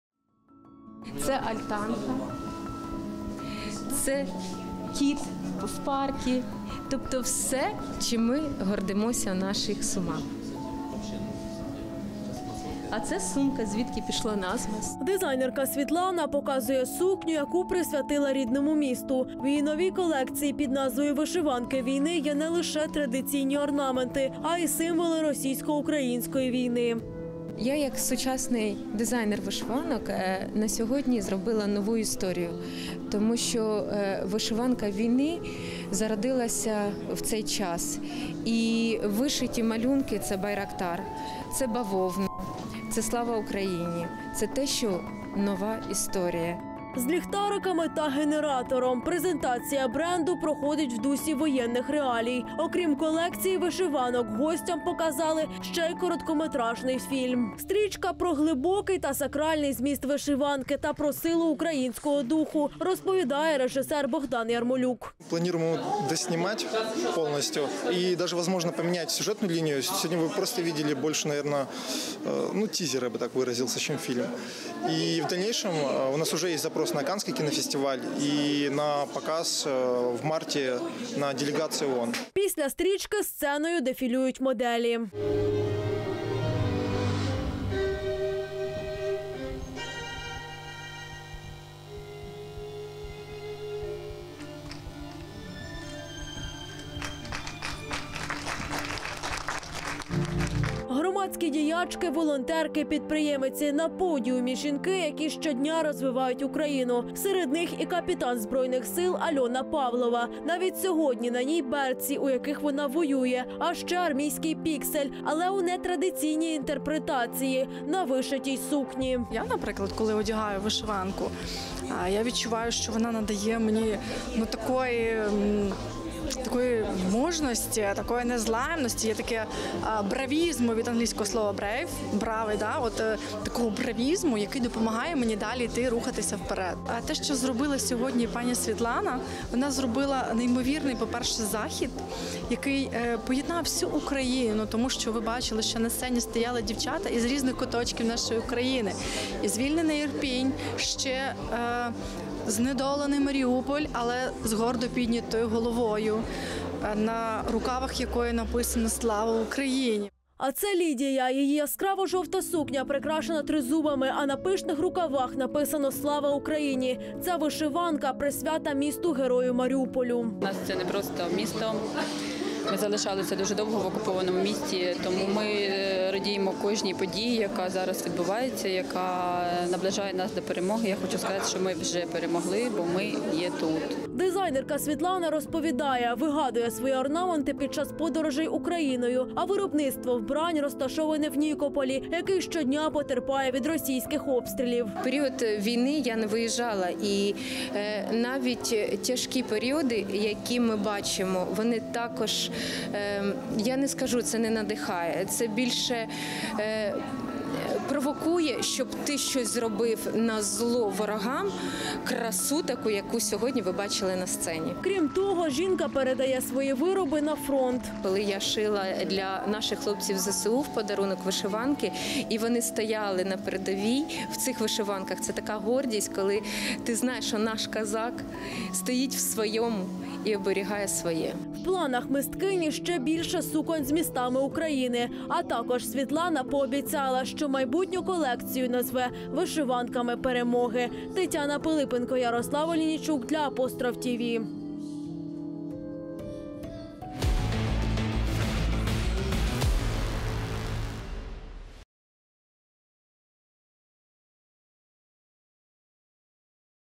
"Байрактари", бавовна та патріотичні написи – українська дизайнерка створює сучасні вишиванки
В ефірі: "Байрактари", бавовна та патріотичні написи – українська дизайнерка створює сучасні вишиванки